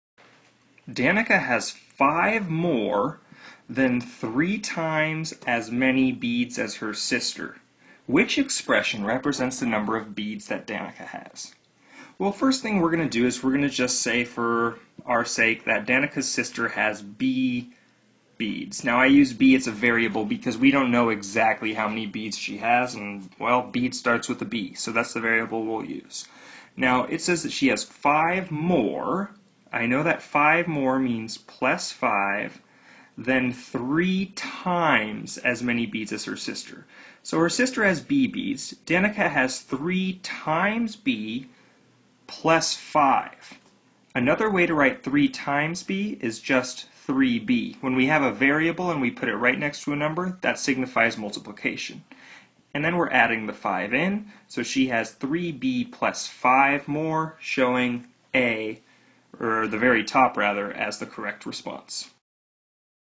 [6.EE.6-1.0] Use Variables in Expressions - Common Core Standard
Use variables to represent numbers and write expressions when solving a real-world or mathematical problem; understand that a variable can represent an unknown number, or, depending on the purpose at hand, any number in a specified set.